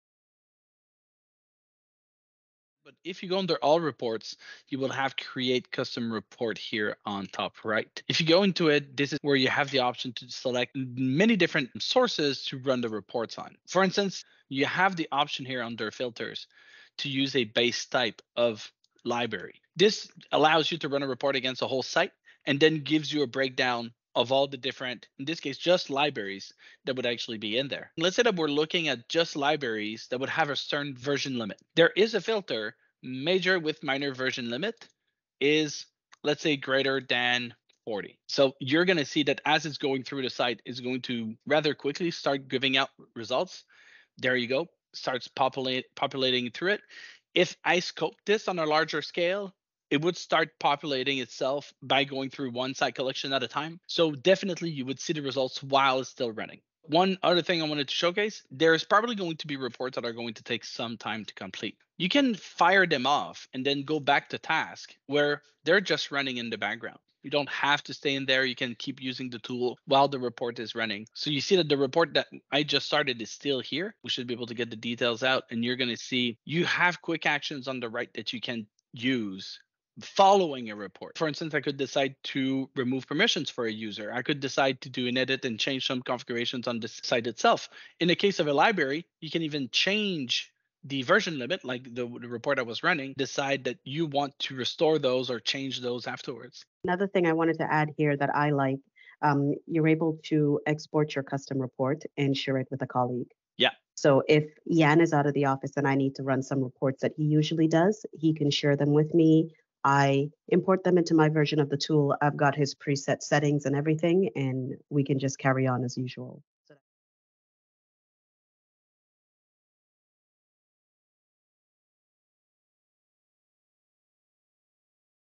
Run Custom Reports on your SharePoint Environment
Run multiple reports to gain insights on your SharePoint and Microsoft 365 environments.

Learn more about ShareGate, your true out-of-the-box management solution for your big migrations and your everyday Teams and SharePoint operations